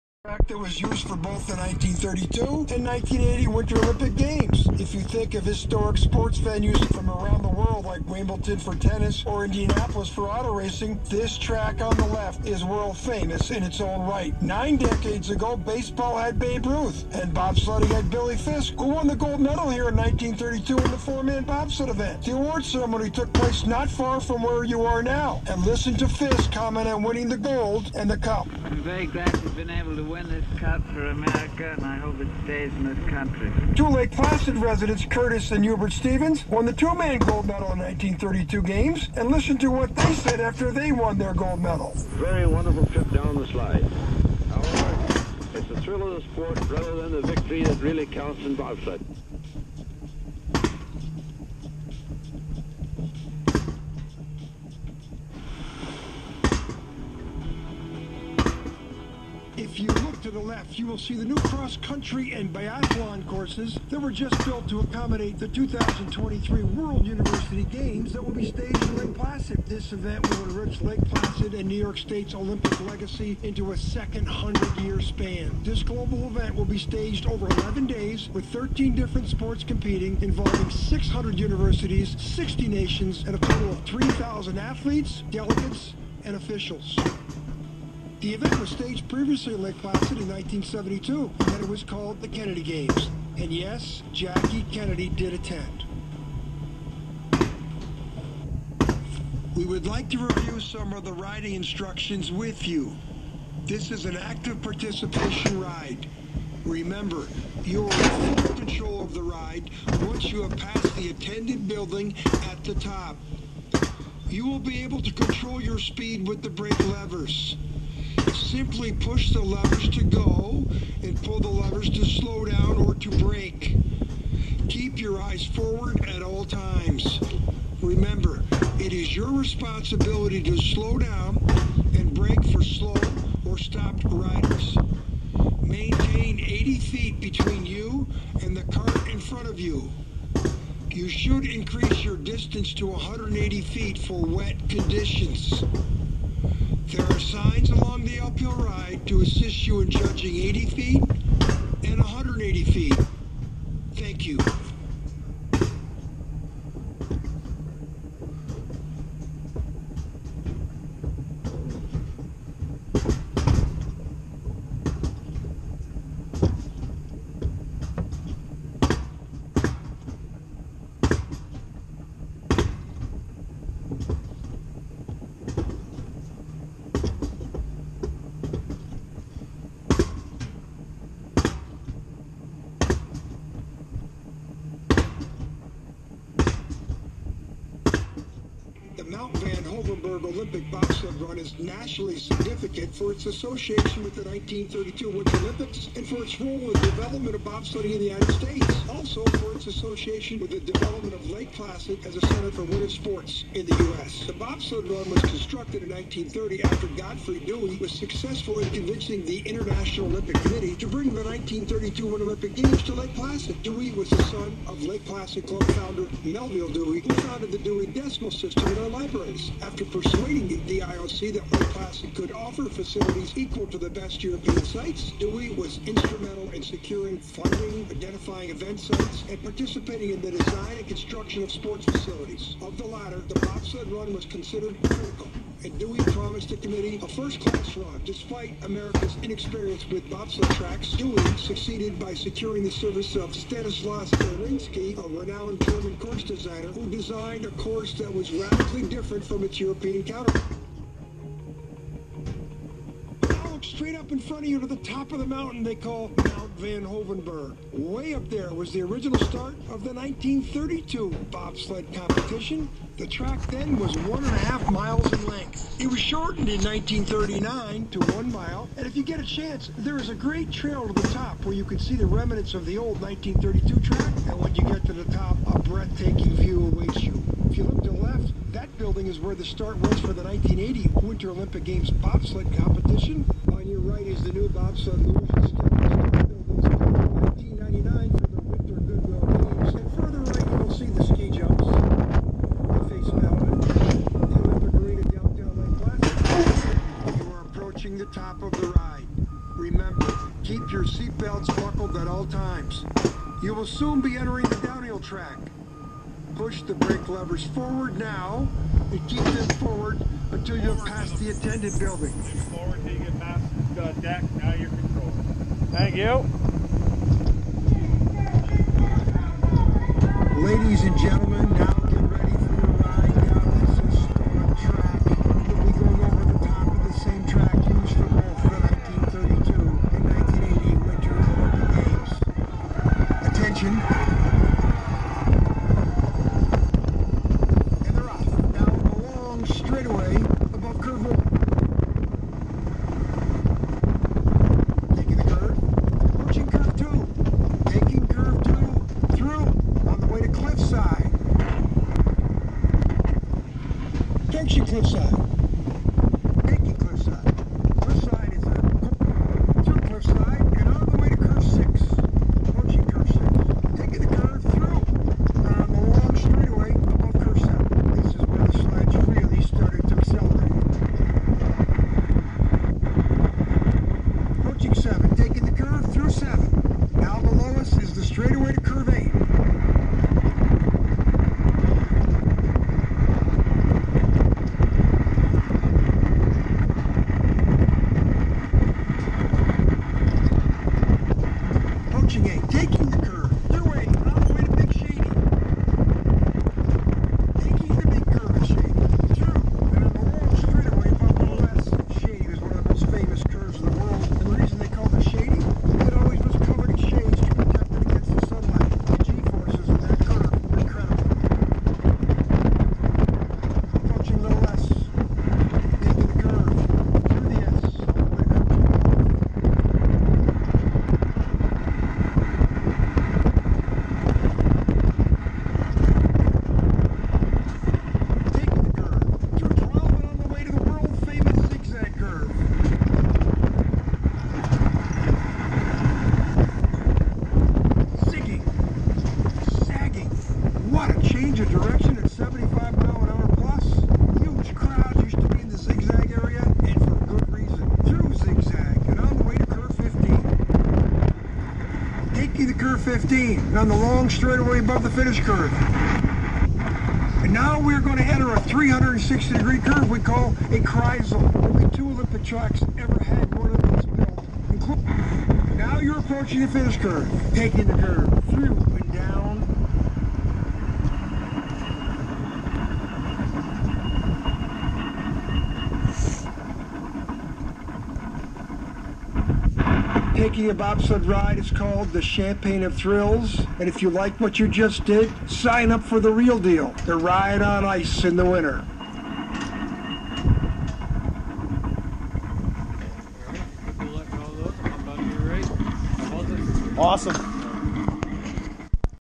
Lake Placid Mountain Coaster 10-2020
take a ride on the all new mountain coaster that follows the 1980 winter olympic bobsled course